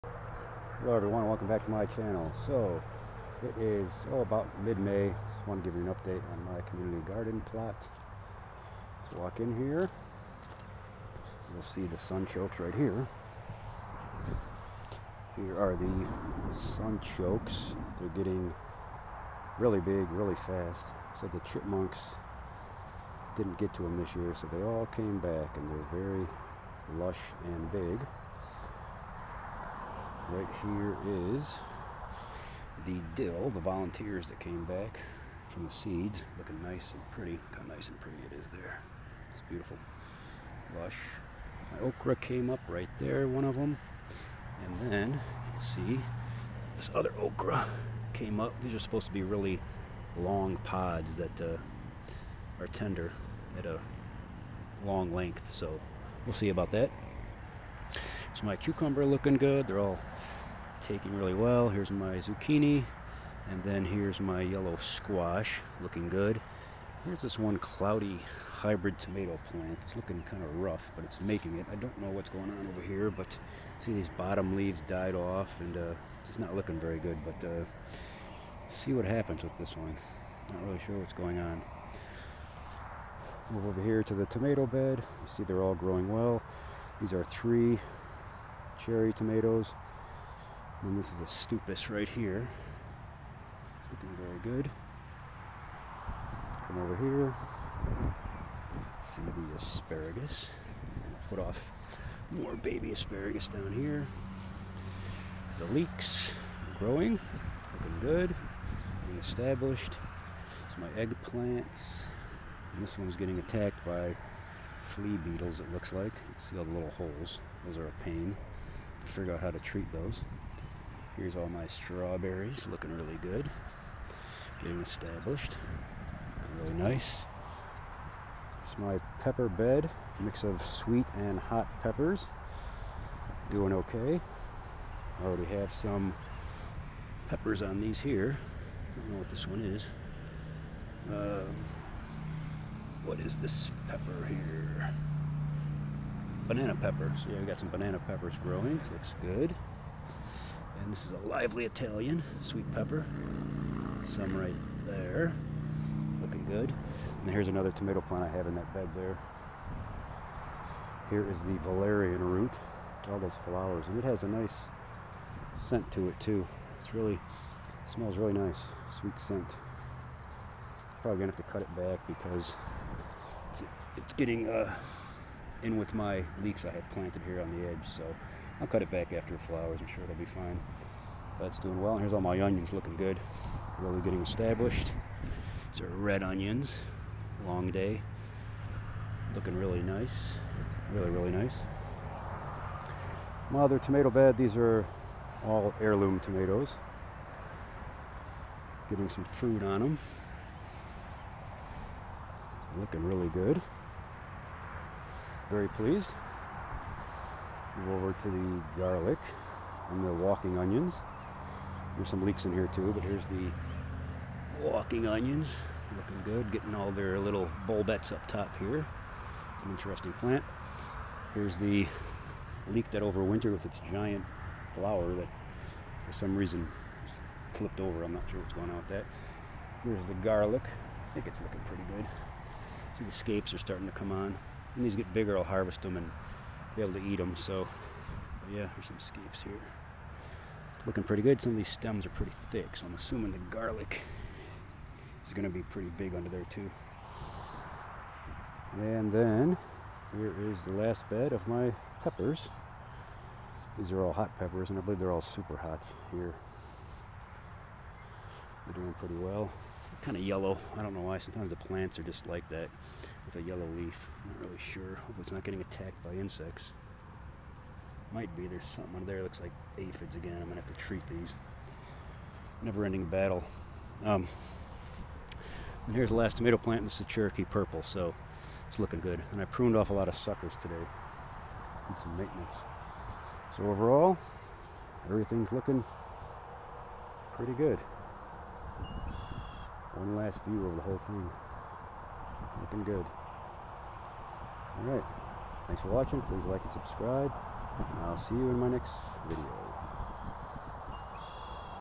Community Garden Plot Update in Mid-June
I provide an update on my community garden plot in mid-June. Hot Peppers, tomatoes, strawberries etc.